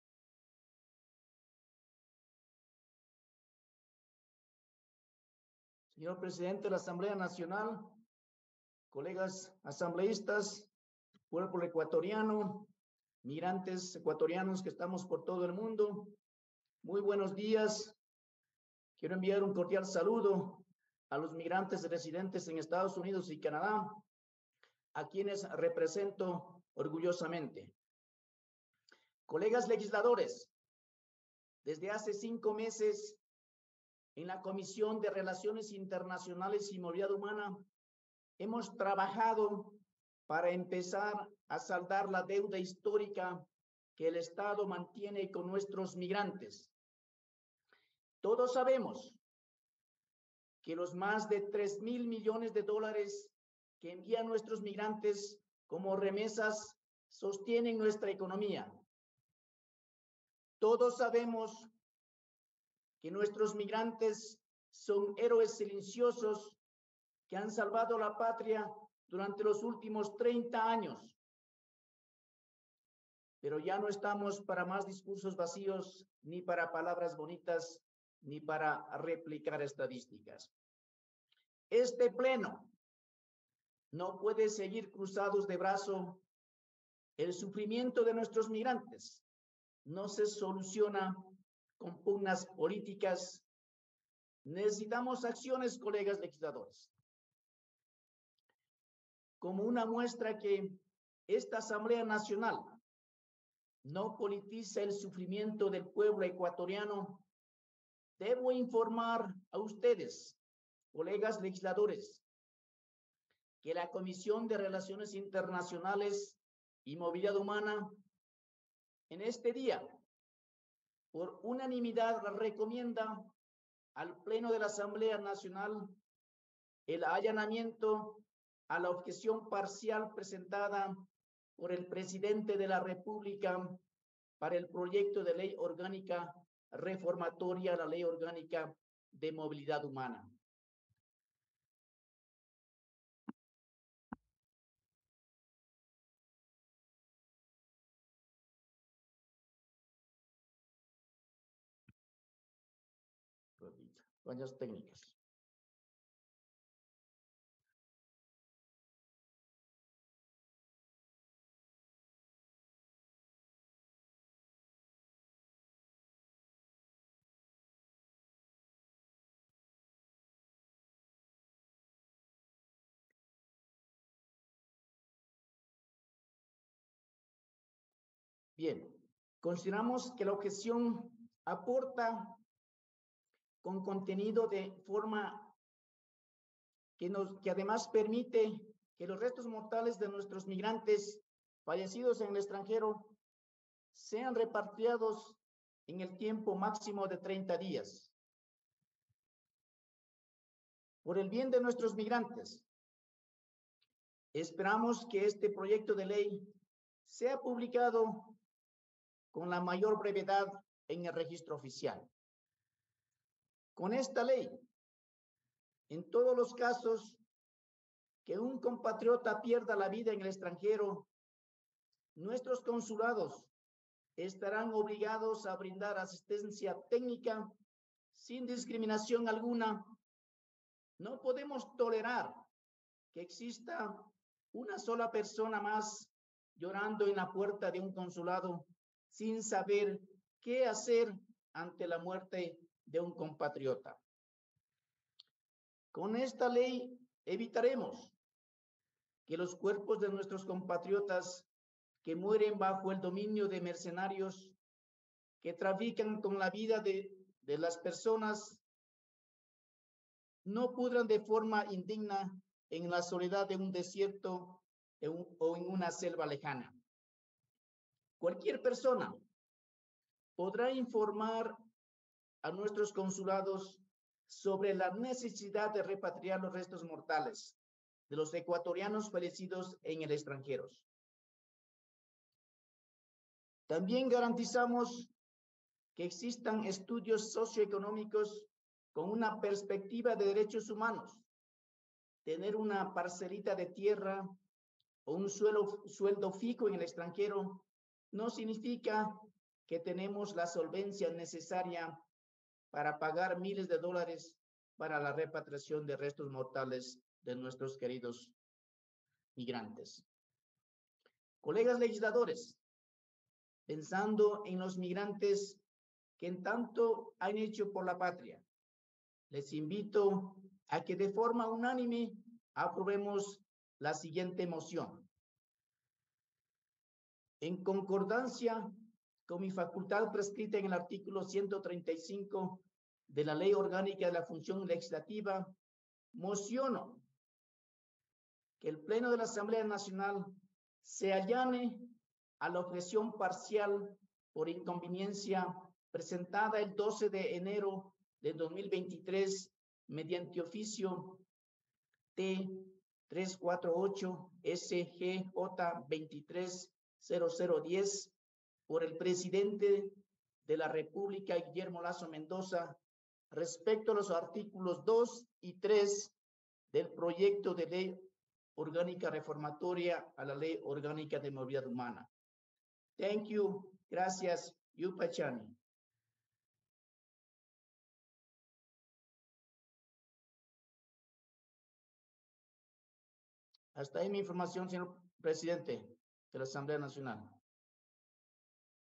Asambleísta Ángel Maita - Sesión 842
31 de enero de 2023
Informe No Vinculante respecto de la Objeción Parcial del Presidente de la República al Proyecto de Ley Orgánica Reformatoria a la Ley Orgánica de Movilidad Humana.